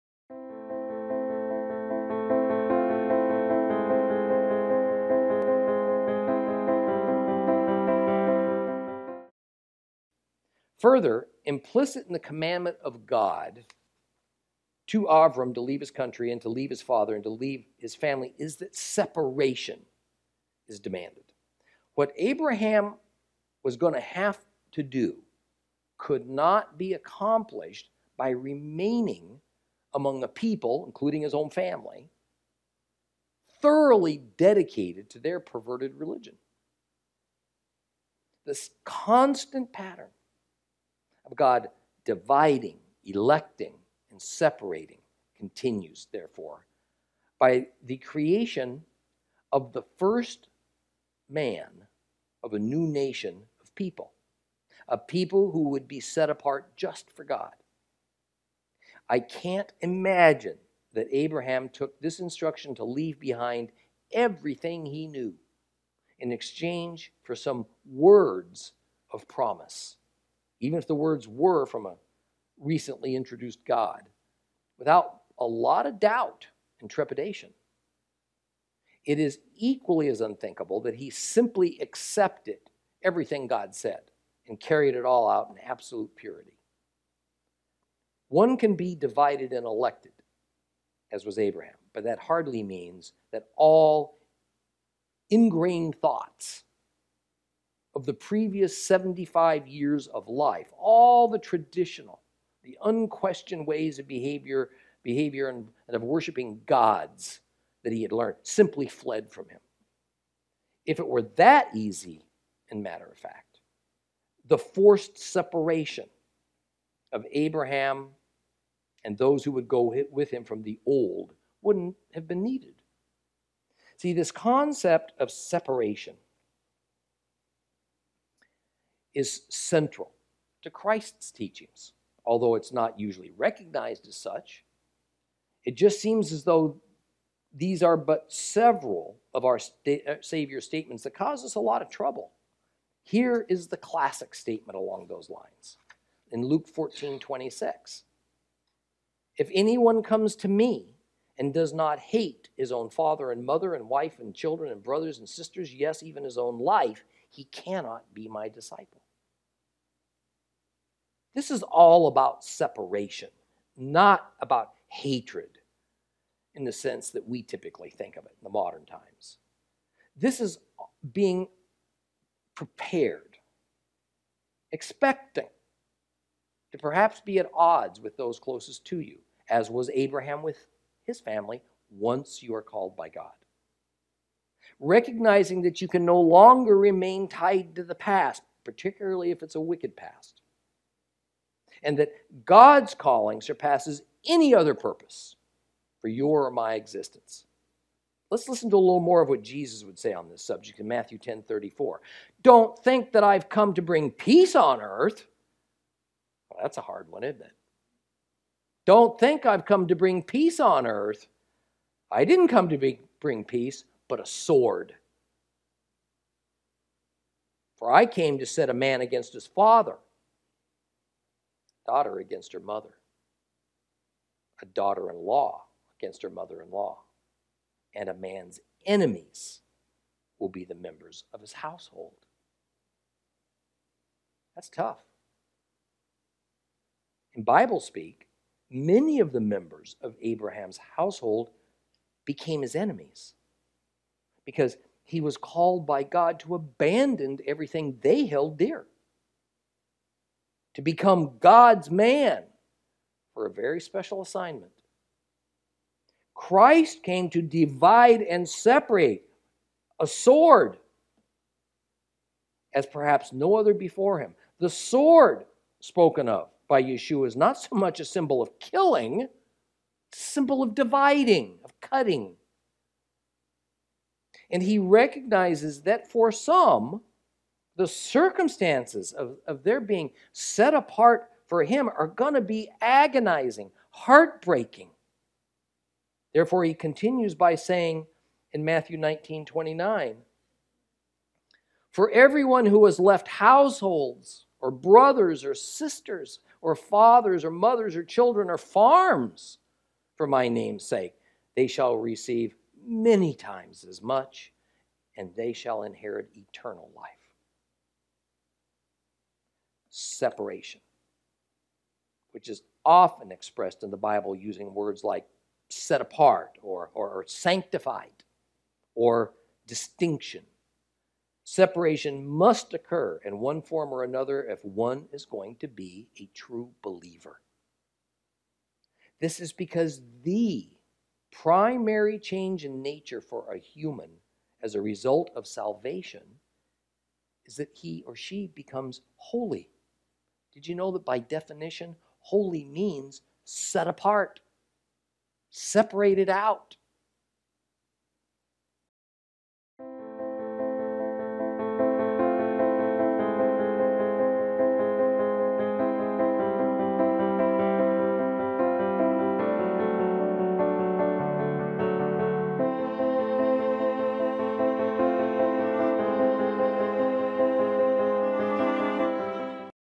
Holiness... Have You Obeyed?
What key action does God require to produce holiness? Tom Bradford elaborates on God's patterns.